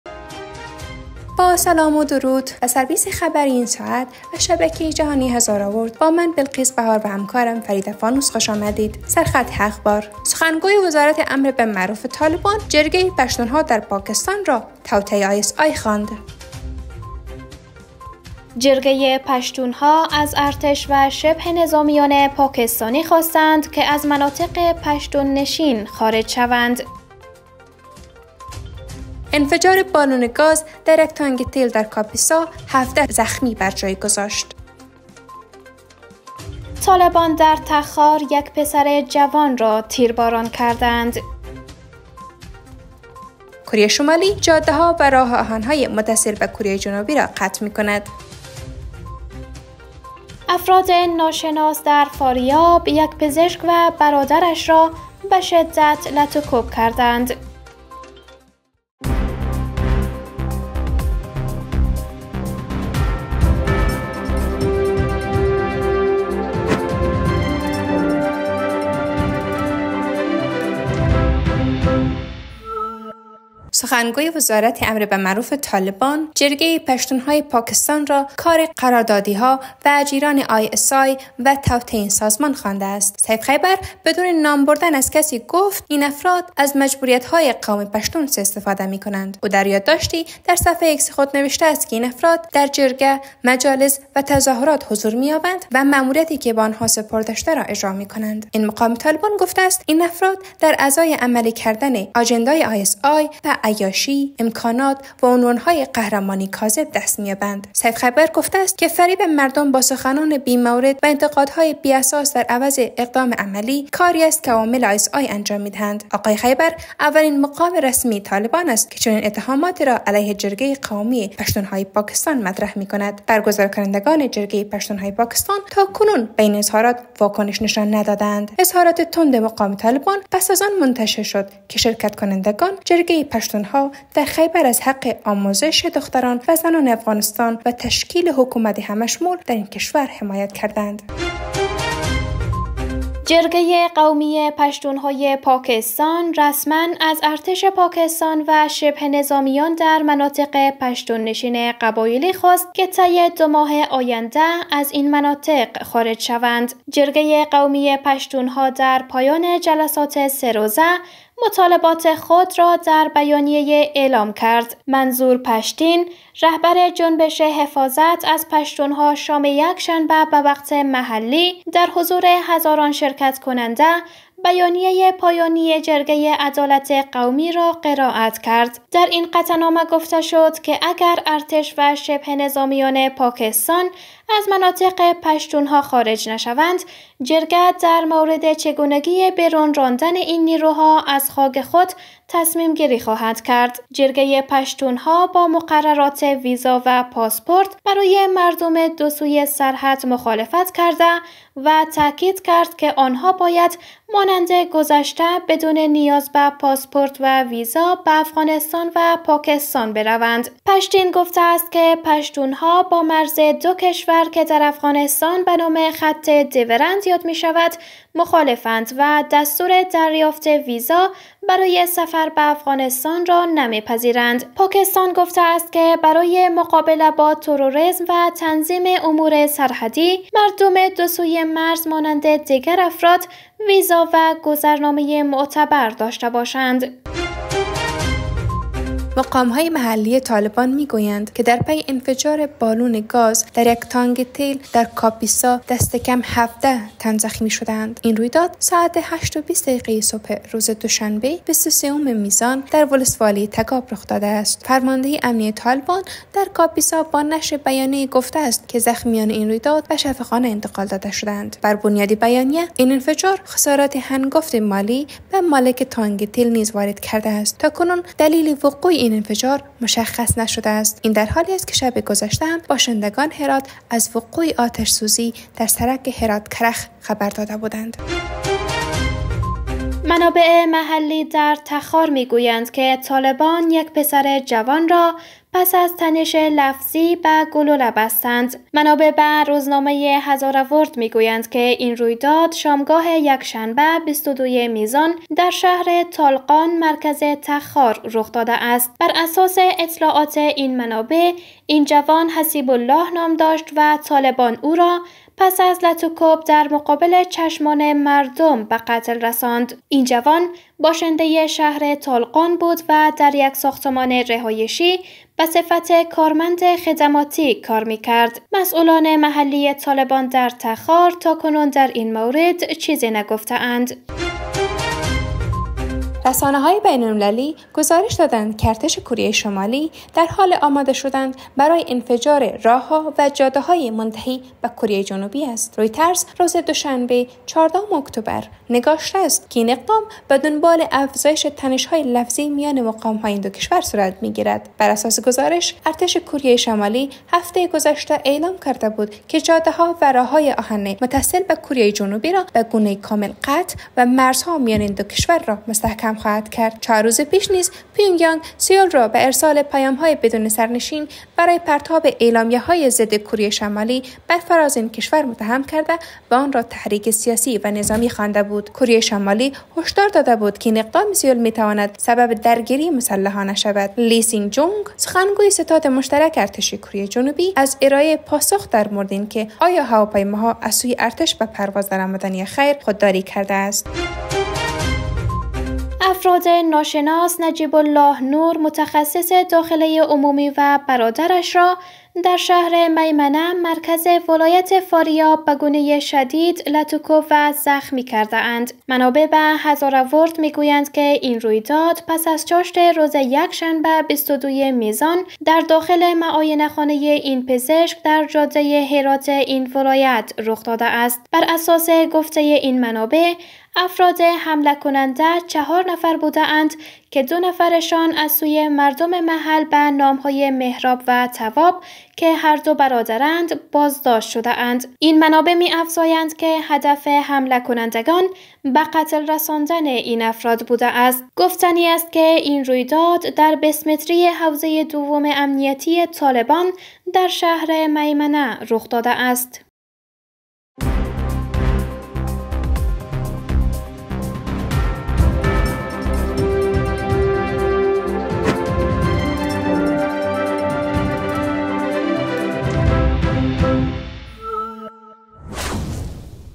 Hazara World 6pm News 14 October خبرهای ساعت۶ هزاره ورلد ۲۳‏ میزان ۱۴۰۳
به خبرهای ساعت 6 هزاره ورلد خوش آمدید. از این پس می توانید مهمترین و تازه‌ترین خبرهای افغانستان، ایران، پاکستان، منطقه و جهان را از این دریچه بیننده باشید.مهمترین خبرهای امروز:

• سخنگوی وزارت امر به معروف طالبان، جرگه پشتون‌ها در پاکستان را «توطئه آی‌اس‌آی» خواند
• جرگه پشتون‌ها از ارتش و شبه‌نظامیان پاکستانی خواست از مناطق پشتون‌نشین خارج شوند
• انفجار بالون گاز در یک تانک تیل در کاپیسا ۱۷ زخمی برجا گذاشت
• طالبان در تخار یک پسر جوان را تیرباران کردند
• کوریای شمالی جاده‌ها و راه‌آهن‌های متصل به کوریای جنوبی را قطع می‌کند
• افراد ناشناس در فاریاب یک پزشک و برادرش را «به شدت لت‌و‌کوب» کردند
لایک و کمنت یادتان نرود.